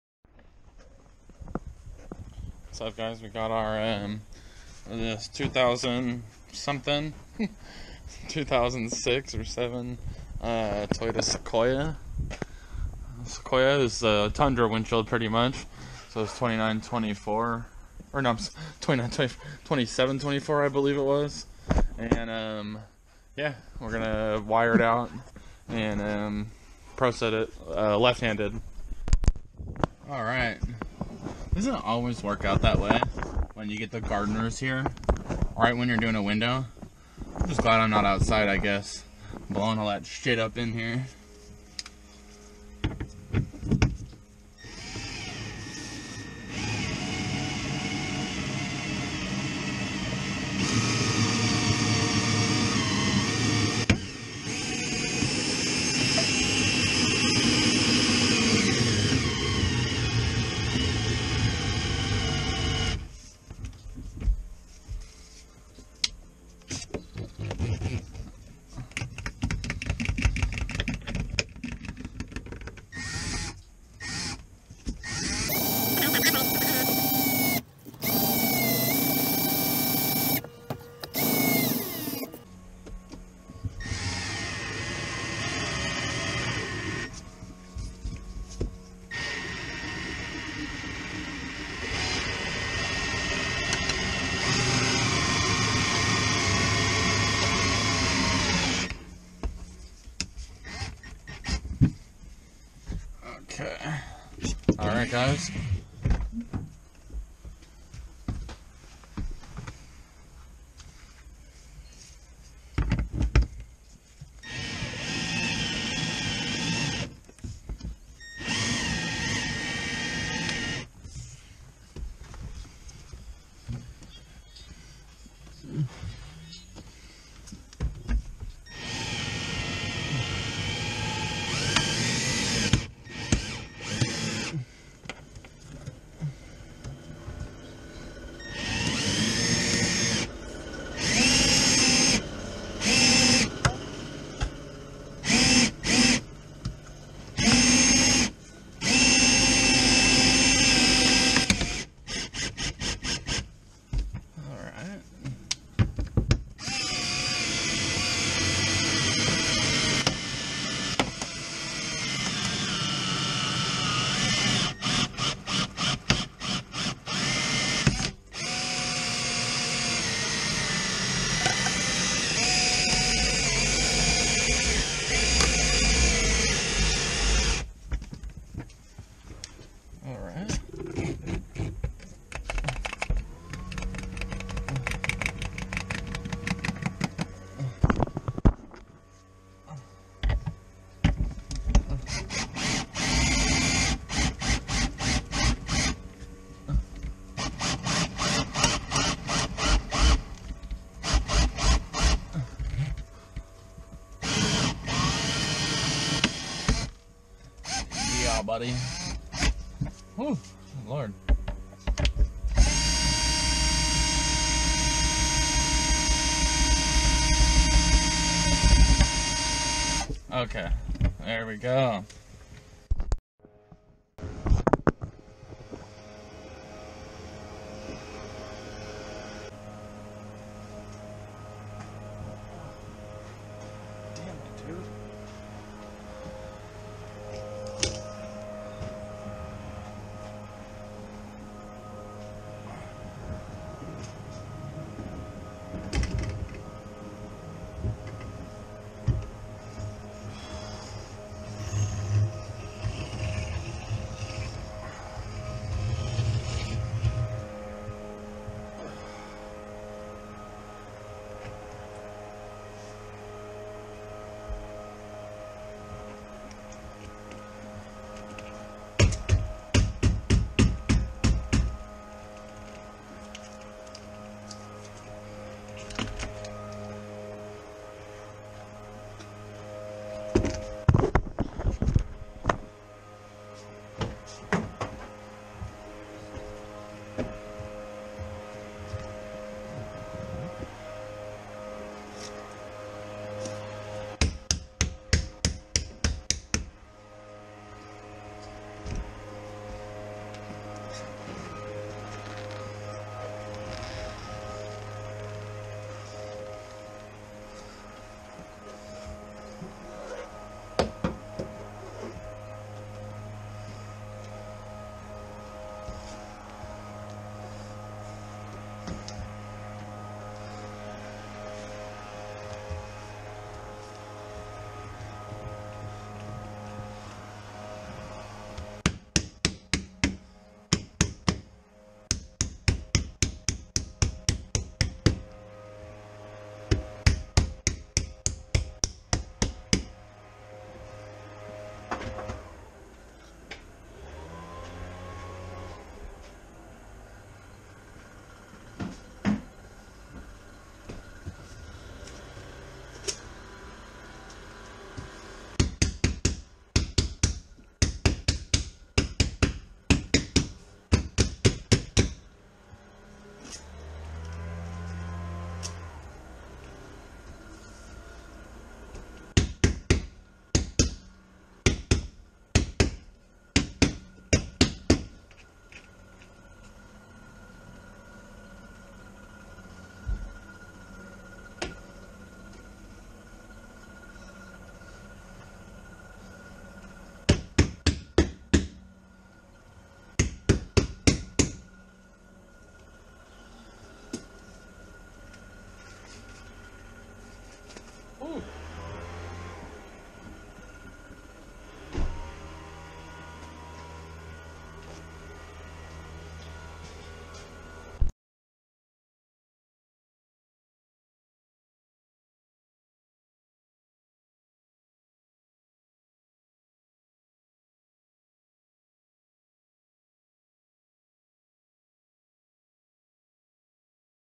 2007 Toyota Sequoia Windshield Replacement ProSet
Hey Peoples here we got a Sequoia in a garage on a rainy day. Im using the Orange Bat for my cutout and then Proset this in with then garage door being low this would make for a complicated hand set.

Let me know what yall think!

WRD Orange Bat at:
SWWRDtools.com